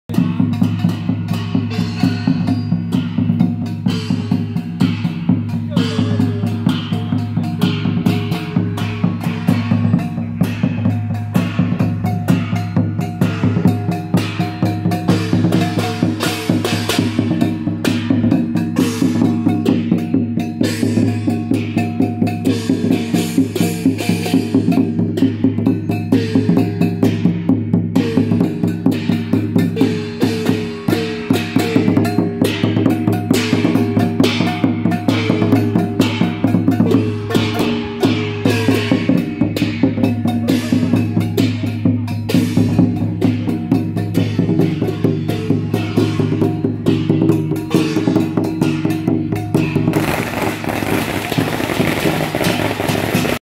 【基隆中正慈惠堂-轎前鼓 接駕 虎尾慈惠堂-眾人員】~111基隆中正慈惠堂登座圓滿暨建堂42週年堂慶大典
基隆中正慈惠堂-轎前鼓 接駕 虎尾慈惠堂-眾人員